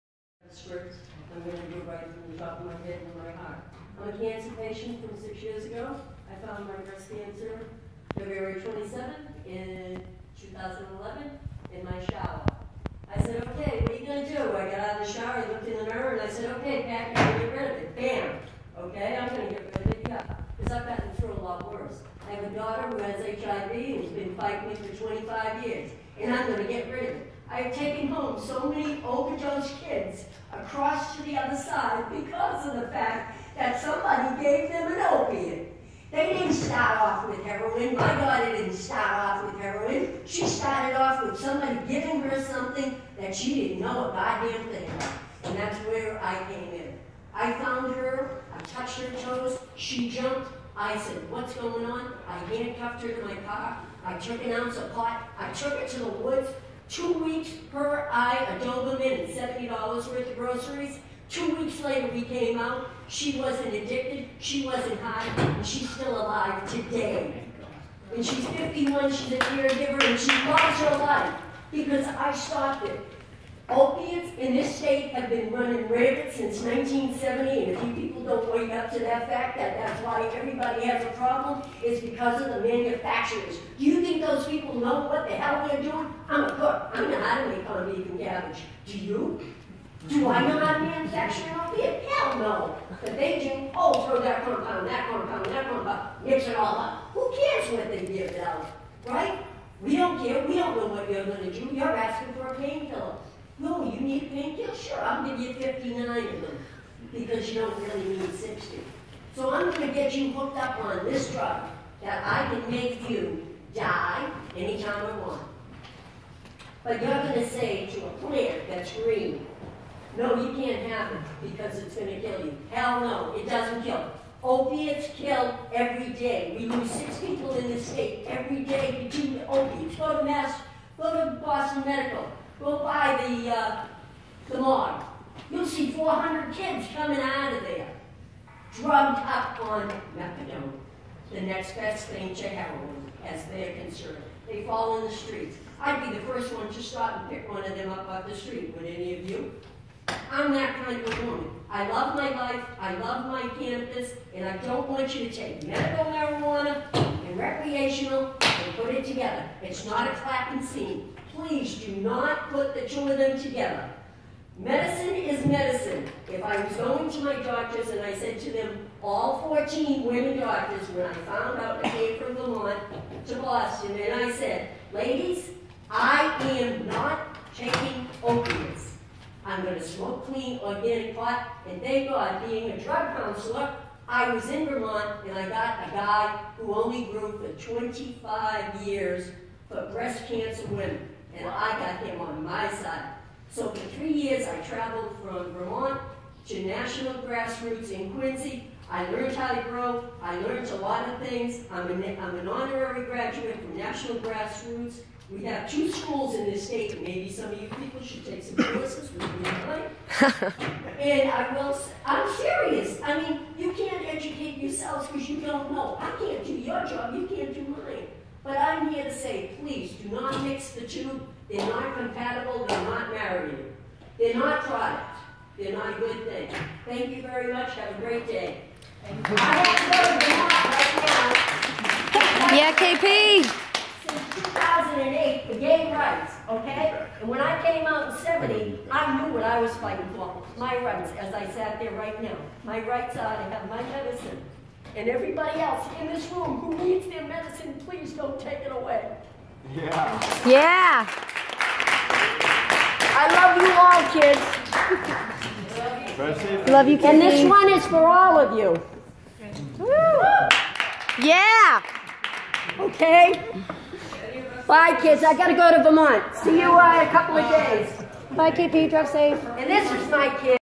KP Owens testifying at the Massachusetts State House
KP Owens testifying to support medical cannabis patients in Massachusetts.